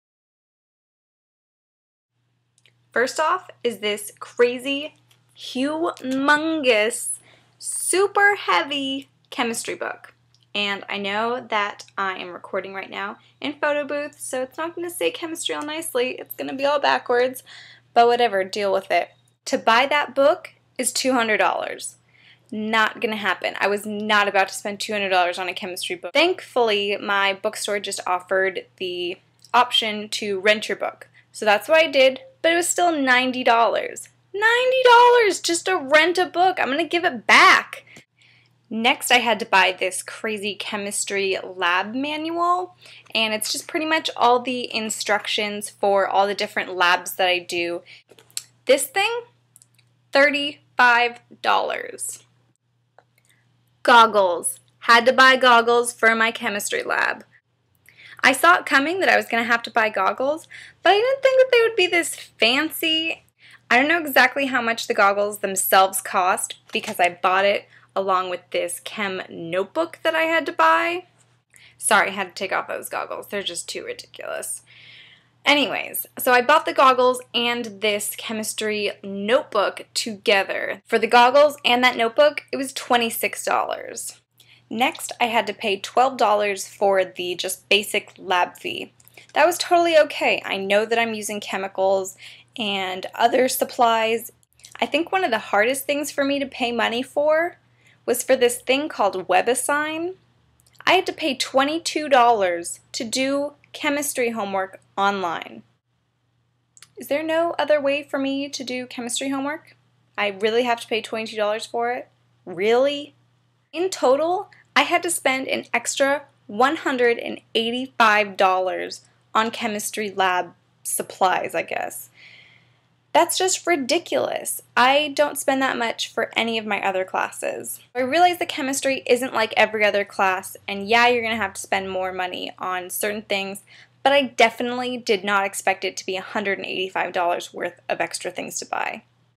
WTF chemistry
EXPAND!!
My break-down of all the things i had to buy for my chemistry class and lab. too many things!